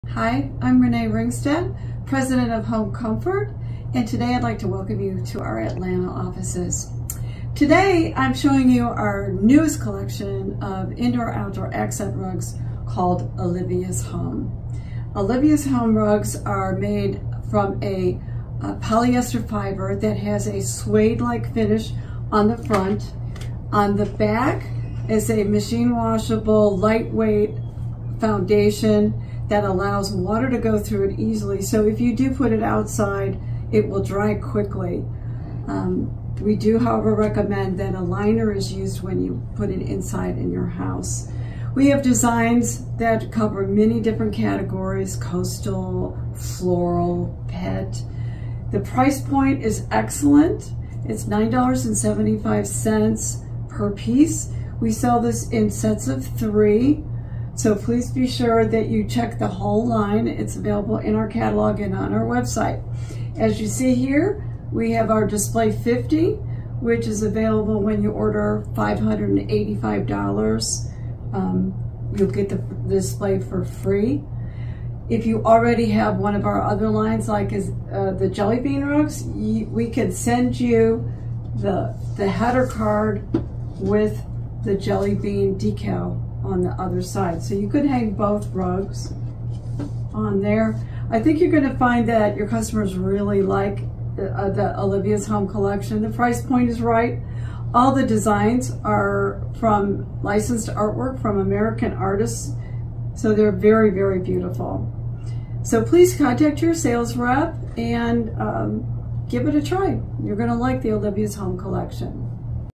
Olivia's Home Rugs
Description of our new washable, fun printed floor mats.